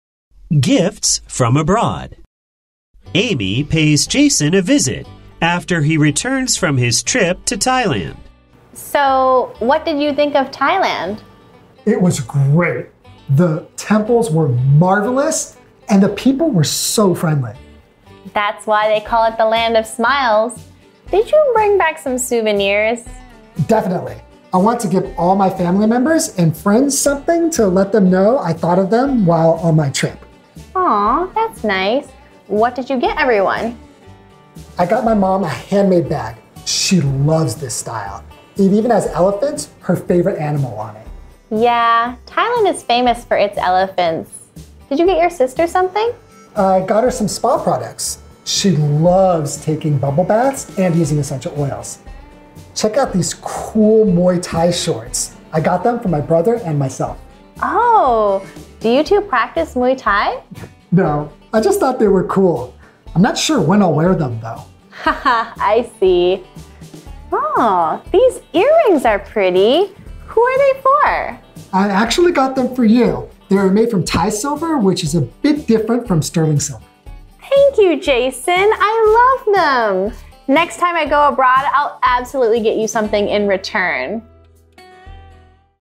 【漂洋過海來送禮】 2023.04.27 Gifts from Abroad
J: Jason A: Amy
Amy pays Jason a visit after he returns from his trip to Thailand.
A: So, what did you think of Thailand?
J: It was great! The temples were marvelous and the people were so friendly.
A: That’s why they call it the Land of Smiles. Did you bring back some souvenirs?
J: Definitely. I want to give all my family members and friends something to let them know I thought of them while on my trip.
A: Aw, that’s nice. What did you get everyone?
J: I got my mom a handmade bag. She loves this style. It even has elephants, her favorite animal, on it.
A: Yeah. Thailand is famous for its elephants. Did you get your sister something?
J: I got her some spa products. She loves taking bubble baths and using essential oils. I got them for my brother and myself.
A: Oh? Do you two practice Muay Thai?
J: No, I just thought they were cool. I’m not sure when I’ll wear them, though.
A: Haha, I see. These earrings are pretty! Who are they for?
J: I actually got them for you. They’re made of Thai silver, which is a bit different from sterling silver.
A: Thank you, Jason. I love them! Next time I go abroad, I’ll absolutely get you something in return.